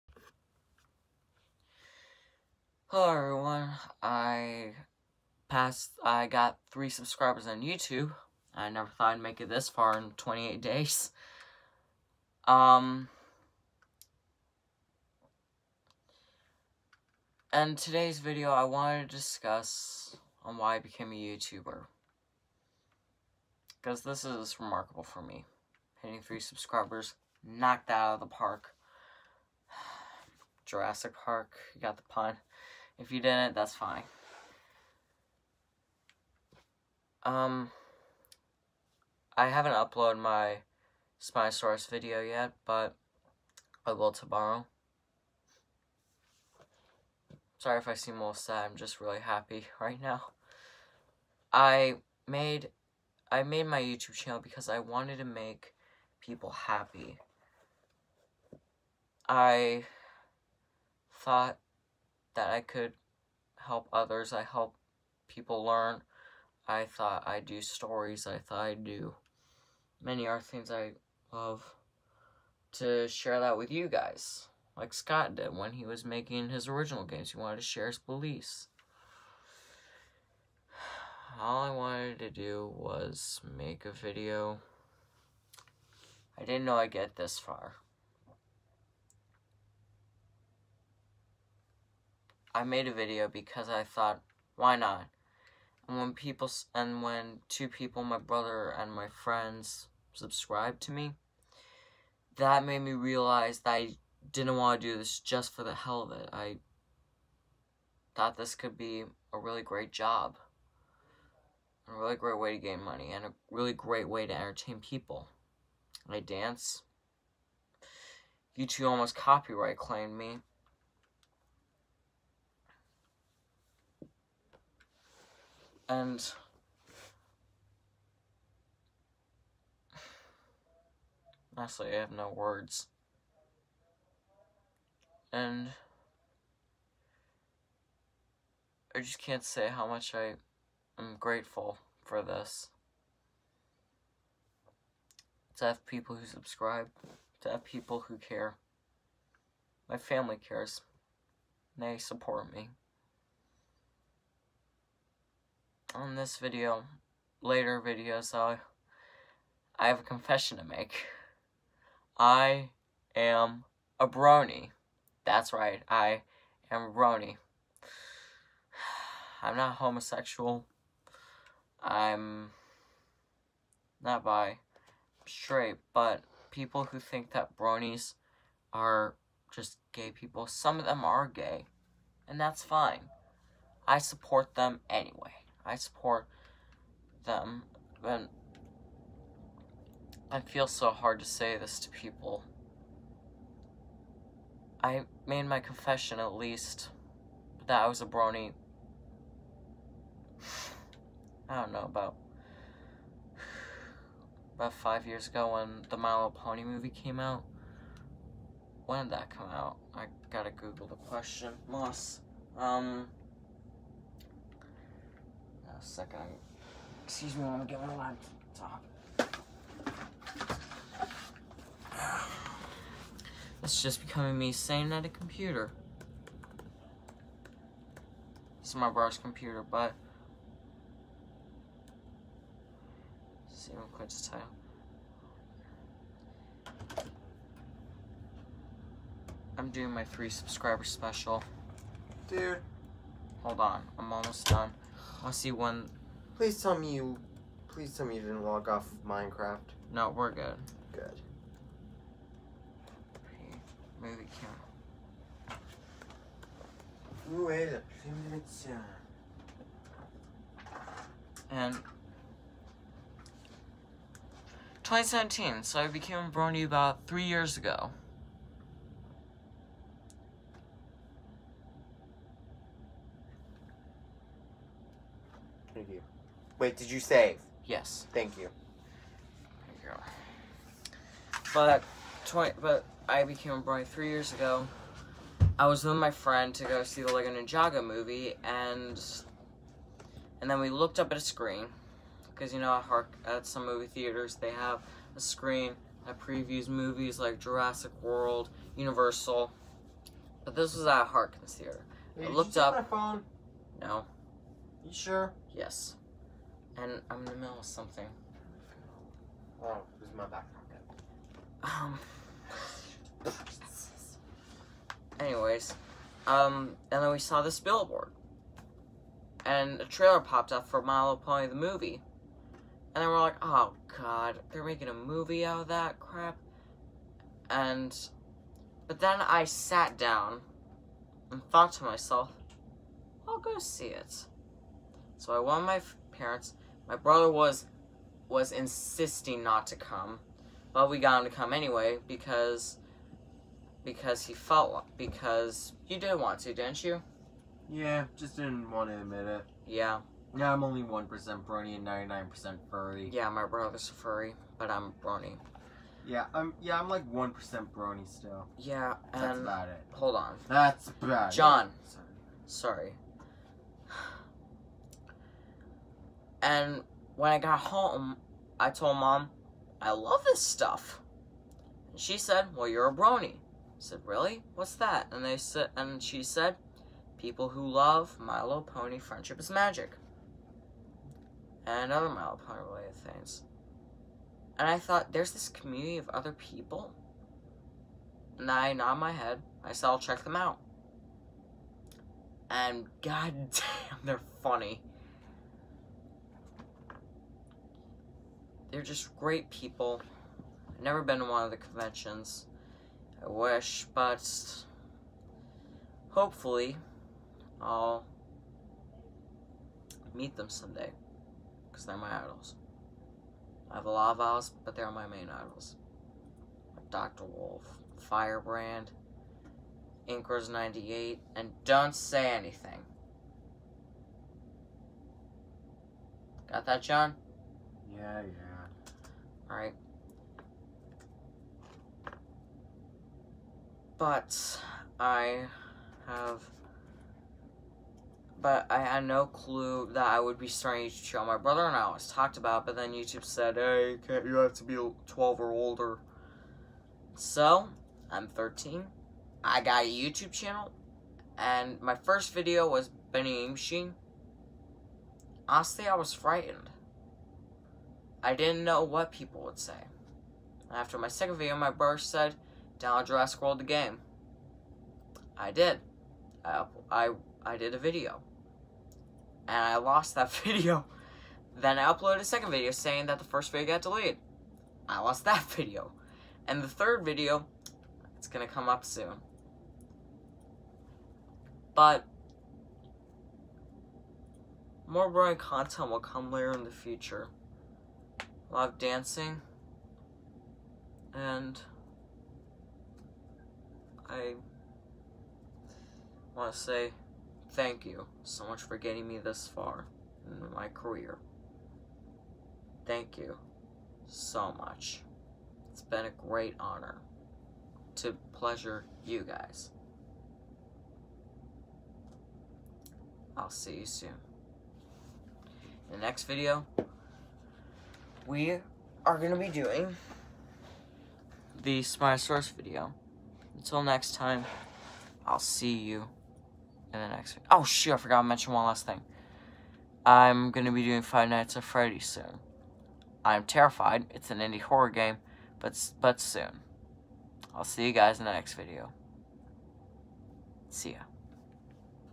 3 subscribers special (Thank you so much)
In this video I surpassed 3 subscribers on YouTube and in this video I have a confession to make